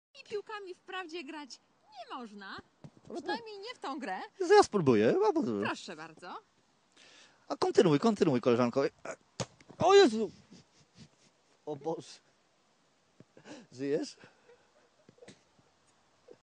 TV Host Accidentally Hits Female Co-Host in the Head With Golf Club
Just two Polish television personalities messing around at a driving range with a beach ball. What could possibly go wrong? Maybe the blonde stands behind the dude as he hits the beach ball and his follow through on his swing smacks her in the side of the head? Yeah, that was probably the only possible outcome of this situation. We'll call this the Bizarro Elin.